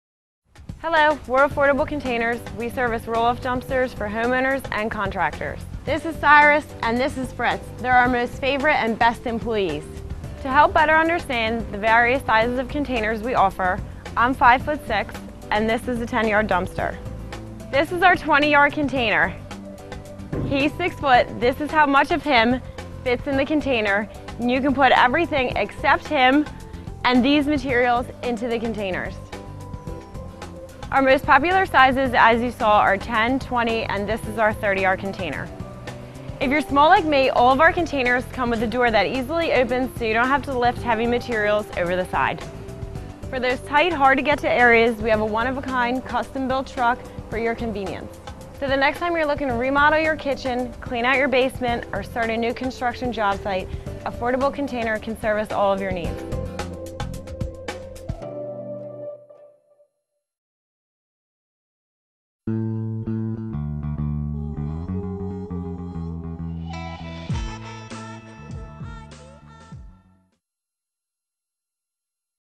Affordable Container Service, LLC | Hauling | Norristown, PA
Affordable Container Service, LLC serves customers throughout Norristown, PA and the surrounding areas. They specialize in dumpster rentals, trash container delivery and pickup, commercial and residential cleanup services, estate cleanouts, home remodeling projects, brush-only containers, commercial building remodels and fit outs, warehouse cleanouts, construction cleanup, demolition debris, custom hauling, new construction job sites, demolition and more.